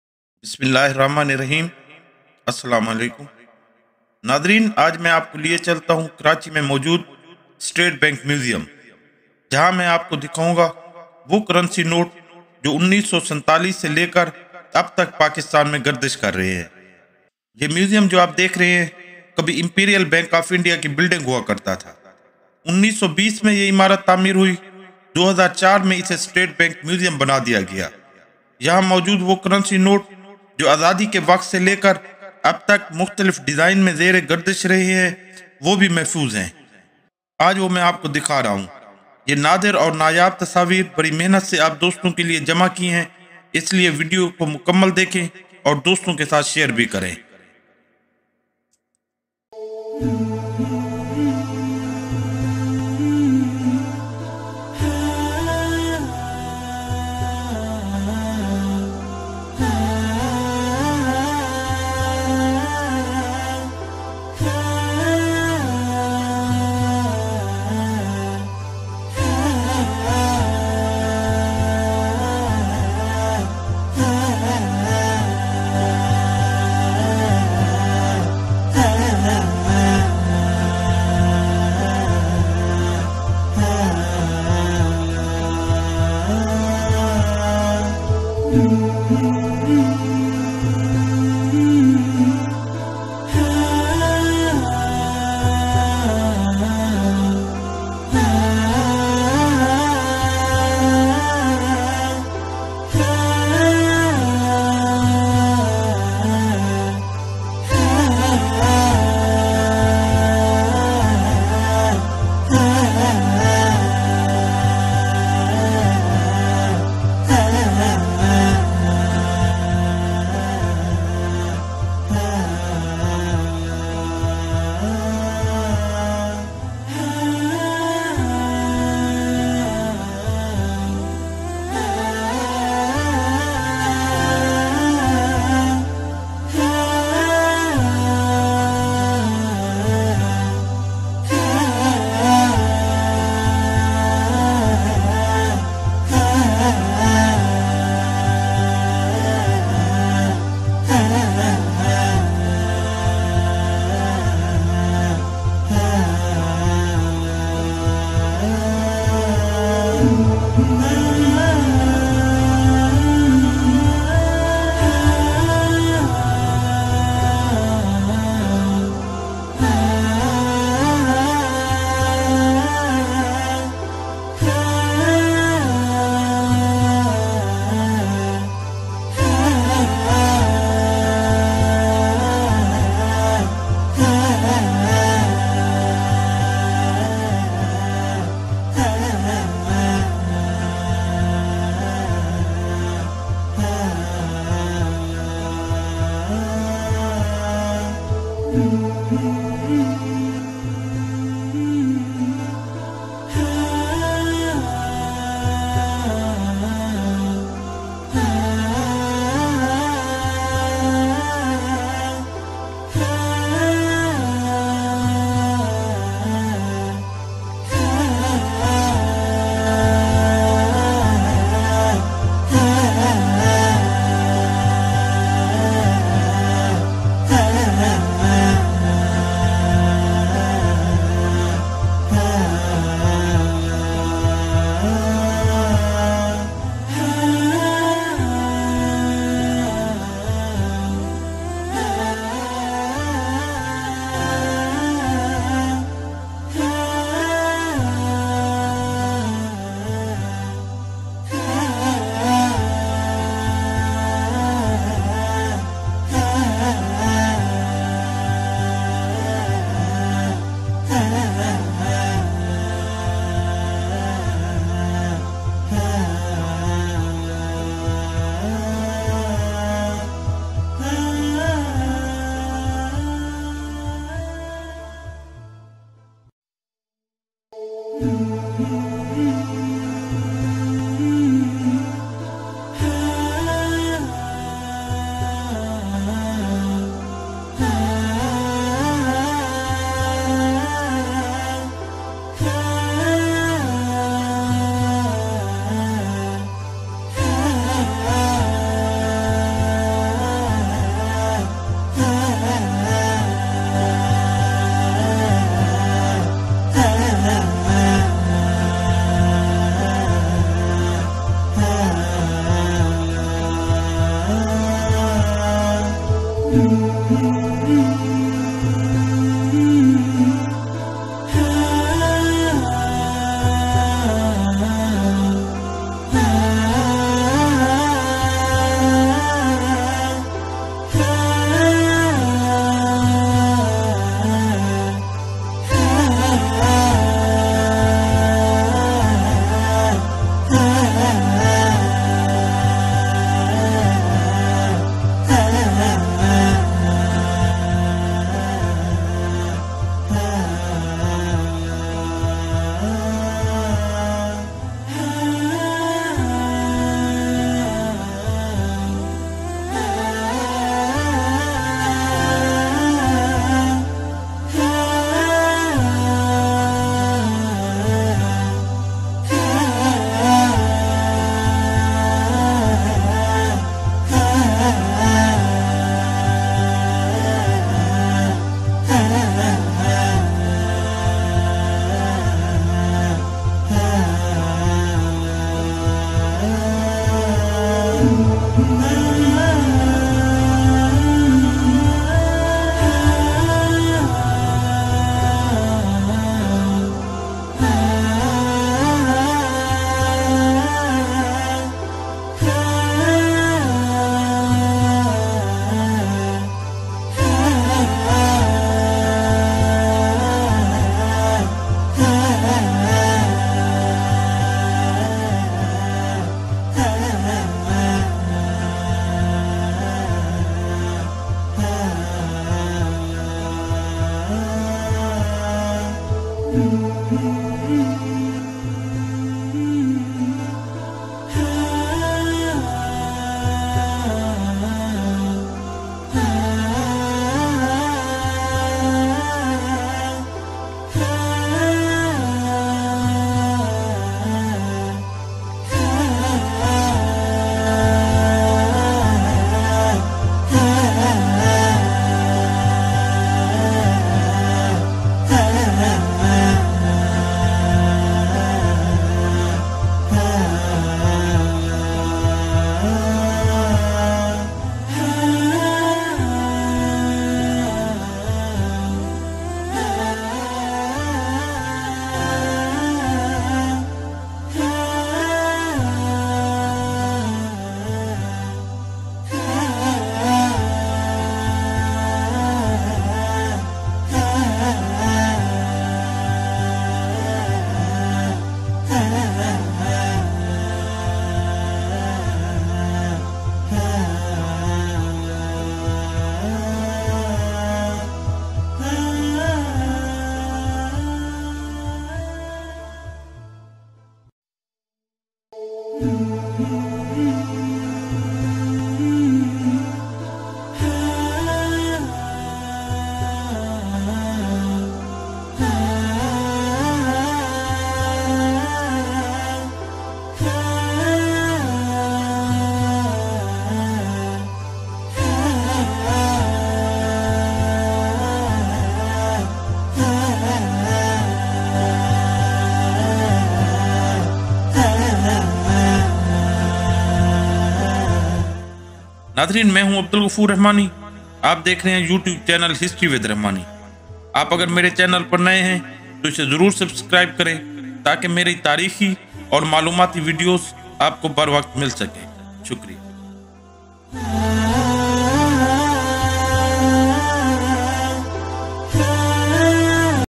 Currency Notes of Pakistan 1947 to 2022 | Rare Images | State Bank of Pakistan Museum
We make videos about History, Culture, Religion, Politics, Autobiographies, and Historical Places